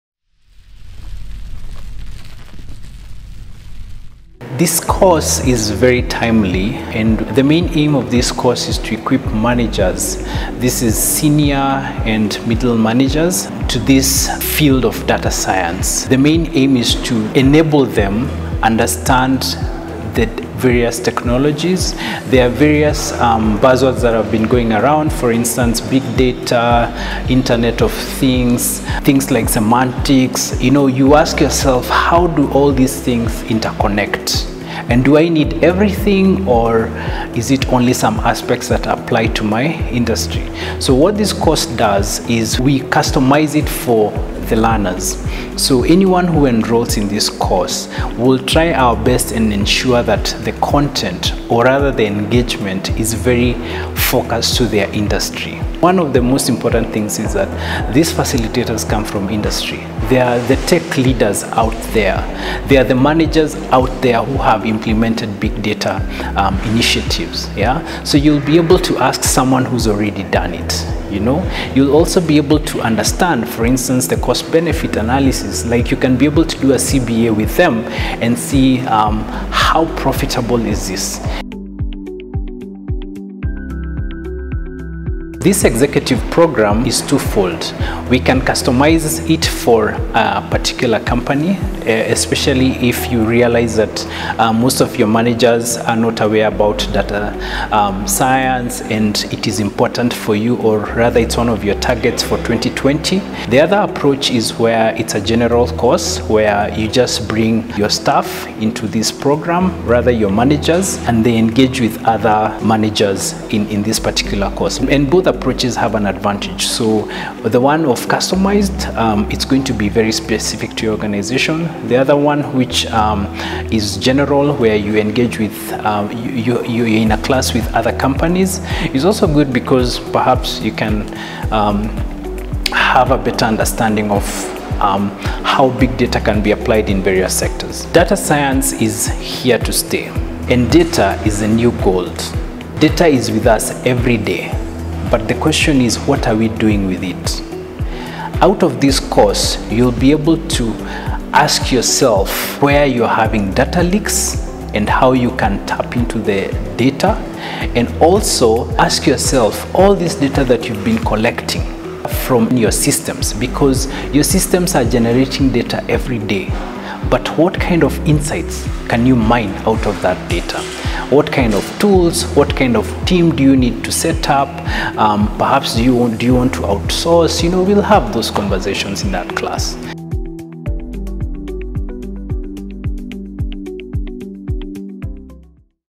Understanding Data Science
The training will cover every step of strategic decision making, with the aim of making optimal use of big-data investments and being able to measure and manage all legal risks that may arise from the use of big-data.